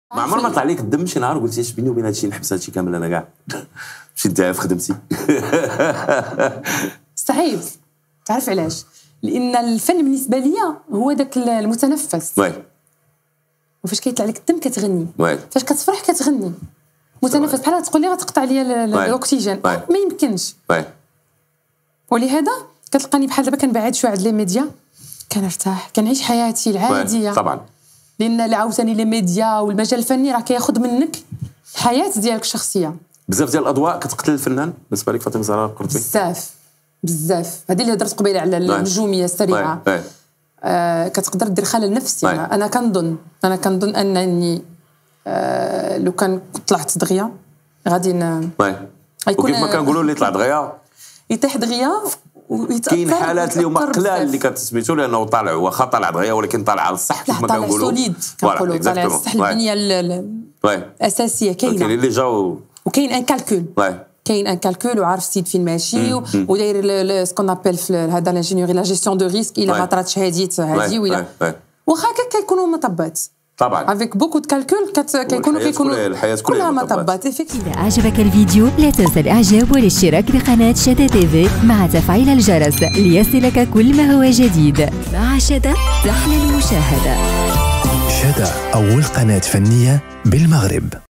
فاطمة الزهراء القرطبي : الفن بالنسبة ليا هو المتنفس و صعيب أنني شي نهار نسمح فيه
CHADA TV | فاطمة الزهراء القرطبي : الفن بالنسبة ليا هو المتنفس و صعيب أنني شي نهار نسمح فيه

1ère Chaîne Artistique au Maroc